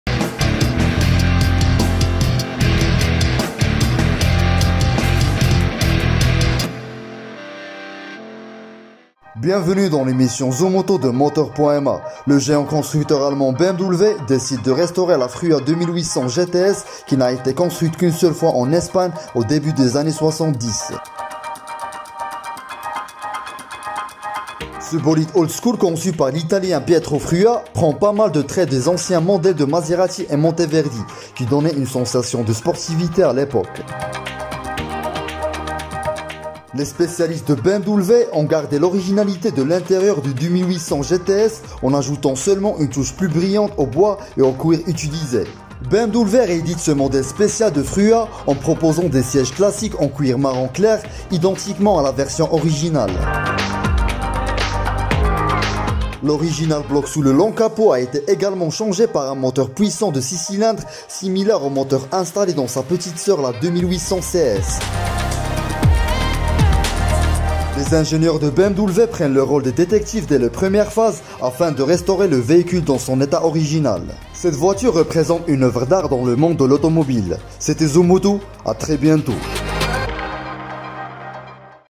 ZOOM AUTO | Bmw restaure la Frua 2800 GTS | 27-03-2020
ZOOM_AUTO : L'actualité automobile par Moteur.ma

Le géant constructeur allemand BMW décide de restaurer la Frua_2800_GTS qui n’a été construite qu’une seule fois en Espagne au début des années 70.

Ce bolide Old school conçu par l'italien Pietro Frua prend pas mal de traits des anciens modèles de Maserati et Monteverdi qui donnaient une sensation de sportivité à l’époque.

Les spécialistes de Bmw ont gardé l’originalité de l’intérieur du 2800 GTS en ajoutant seulement une touche plus brillante au bois et au cuir utilisé.

Bmw réédite ce modèle spécial de Frua en proposant des sièges classiques en cuir marron clair identiquement à la version originale.

L'original bloc sous le long capot a été également changé par un moteur puissant de 6 cylindres similaire au moteur installé dans sa petite soeur la 2800 CS.

Les ingénieurs de Bmw prennent le rôles des détectives dès les premières phases afin de restaurer le véhicule dans son état d'origine.

Cette voiture représente une oeuvre d'art dans le monde de l’automobile !